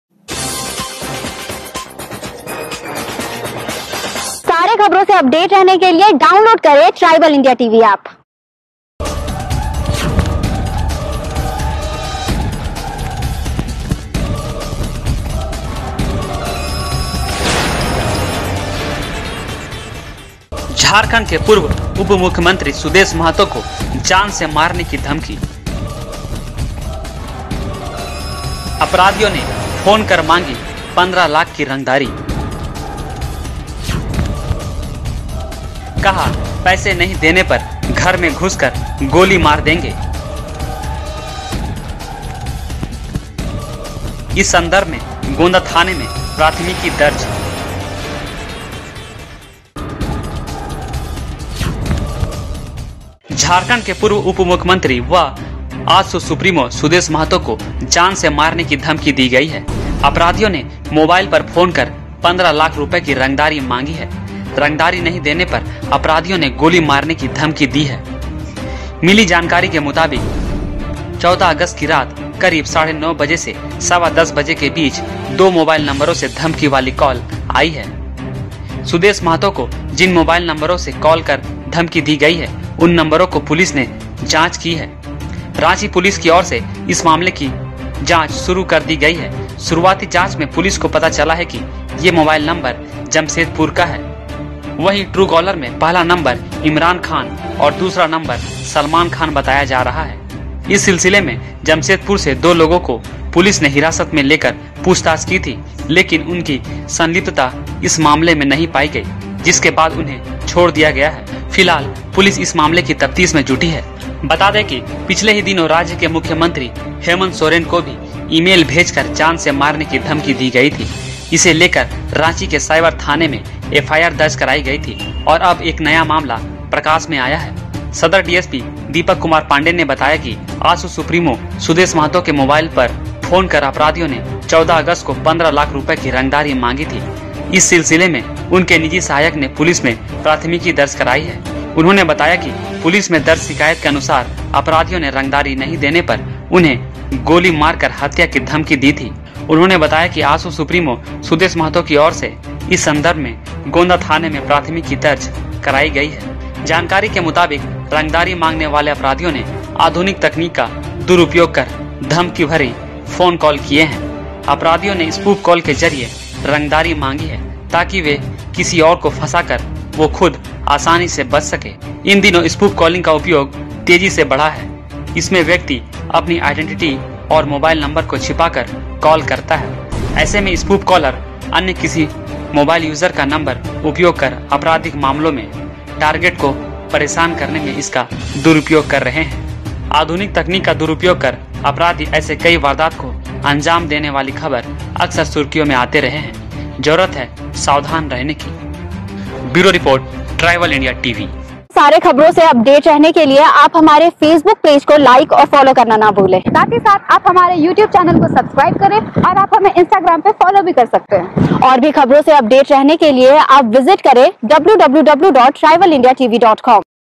jharkhand: पूर्व उप मुख्यमंत्री सुदेश महतो को जान से मारने की धमकी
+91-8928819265

Jharkhand Bureau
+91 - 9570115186